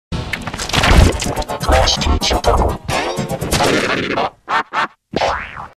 Soviet Cartoon Wolf Hates Klasky Csupo In G-Major 200 (Instructions In Description)
Note: This is the original G-Major 200 made first on Vegas Pro.

Note 2: It started off first with HSL Adjust.

How To Make G-Major 200:

(VideoFX)

1st: HSL Adjust - Hue 0.175/G-Major 200

Add To Hue: 0.175
Saturation: 1.000
Luminance:  1.000

2nd: Mirror - Reflect Left

3rd: Wave - Cry Effect

(AudioFX)

Classic
A17, Drums, unpitched (minimu echo)

-7
-3
0

Effect by Jayden Klapof